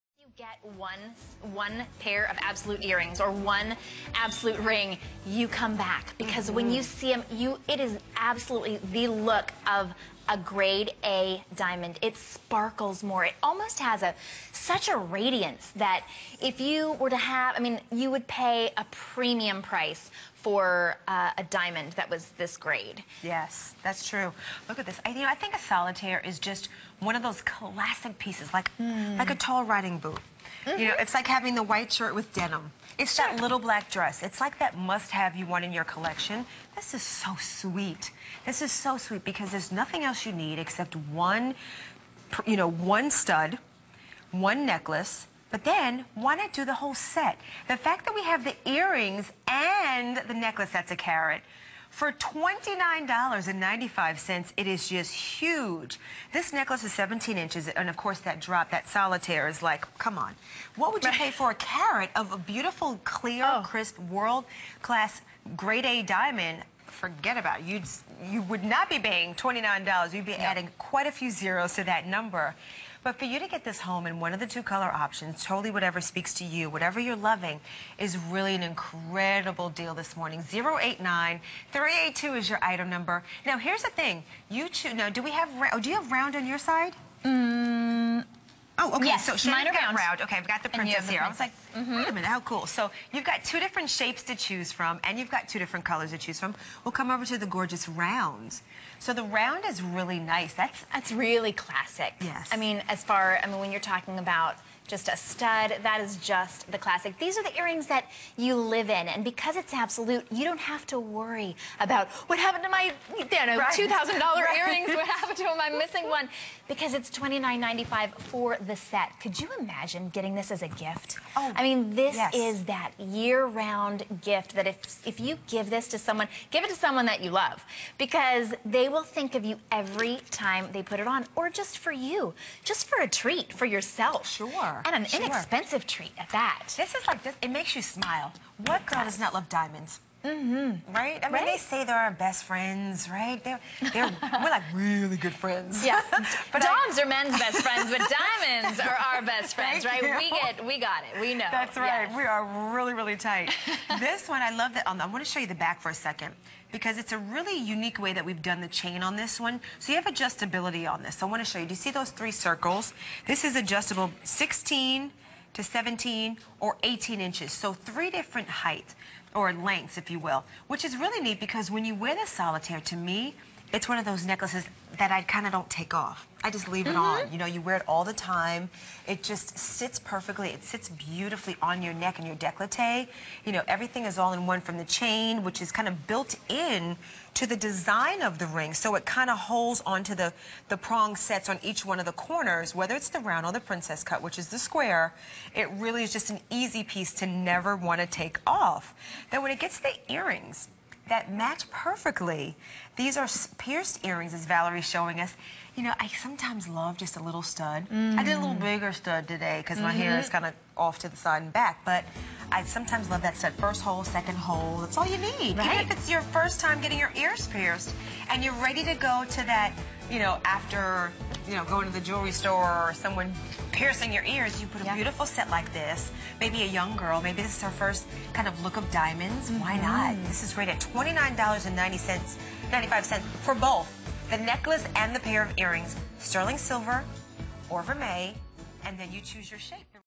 1ct Absolute Round Drop Necklace and Earrings Set
1ct Absolute Round Drop Necklace and Stud Earrings Set
Puttogether pretty. Find that in this perfectly coordinated jewelry set. Its petite Absolutes create a sweet look that twinkles with...